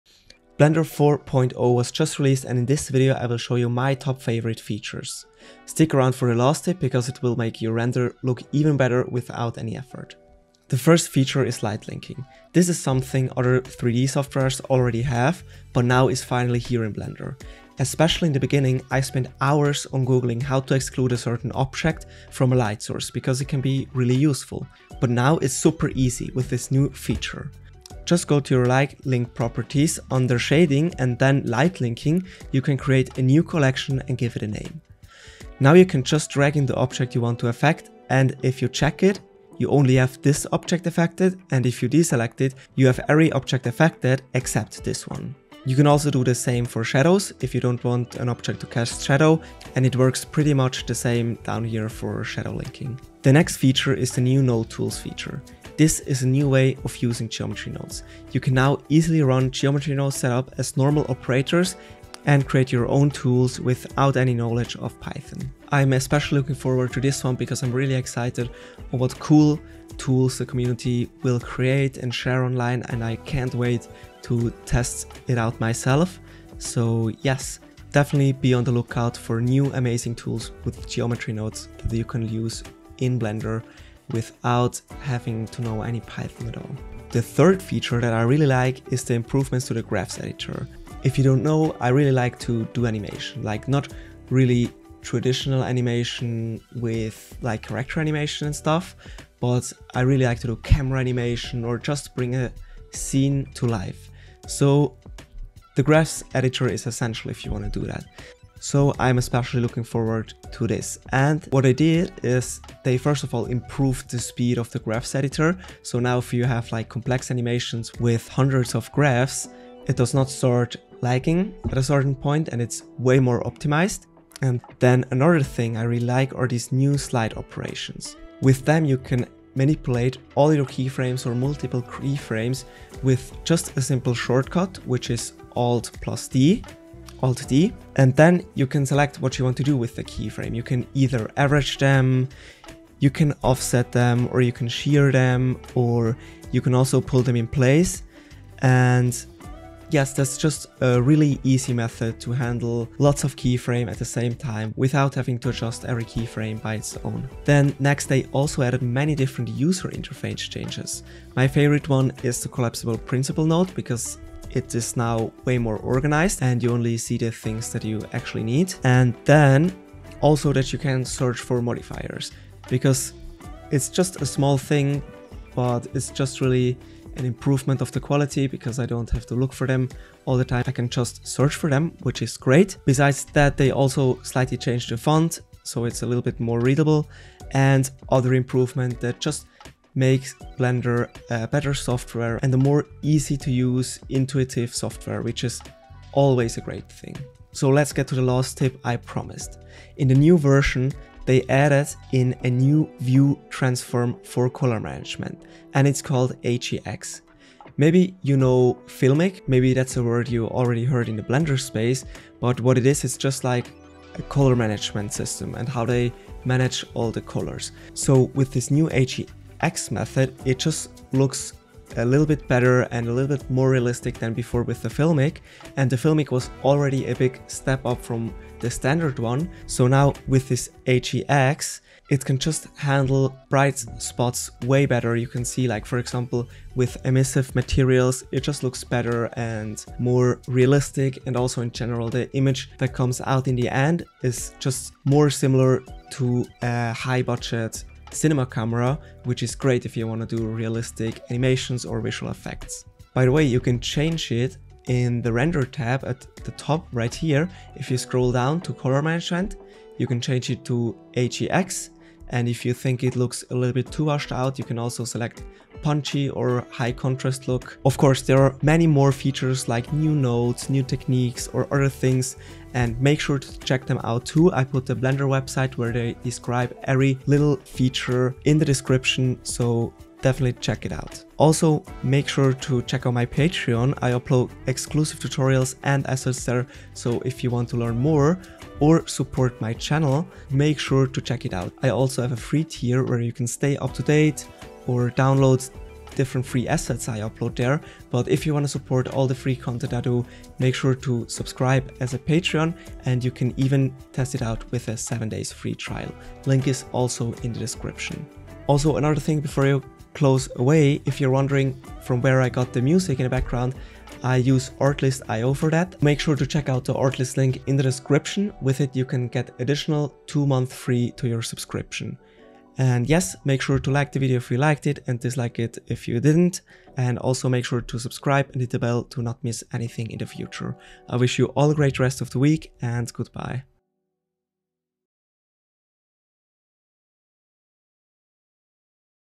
Blender 4.0: Here Are 5 NEW Exciting features!
In this Video I will show you my top 5 favorite new Blender features that come with the new 4.0 Blender version!

Chapters:
00:00 Intro
00:10 Light Linking
00:54 Node Tools
01:28 Graphs Editor
02:38 UI Improvements
03:17 AgX View Transform
04:42 Outro